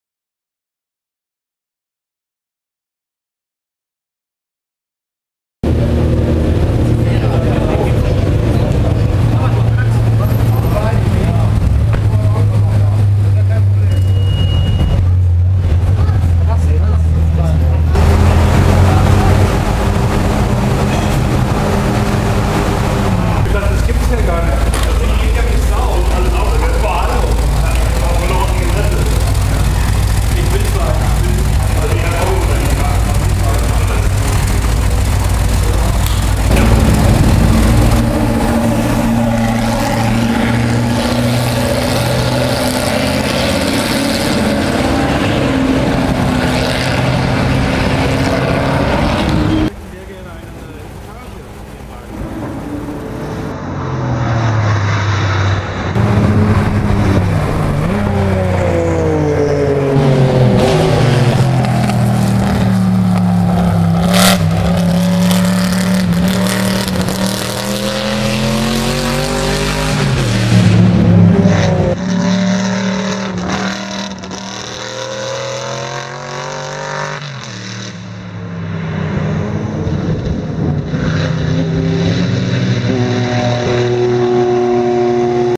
Modena Trackday 2013 @ Francorchamps - (Mercedes) Isdera Imperator 108i
Isdera Imperator 108i (Mercedes) @ Modena Trackday 2013
Nice combo with the Shelby Cobra Daytona!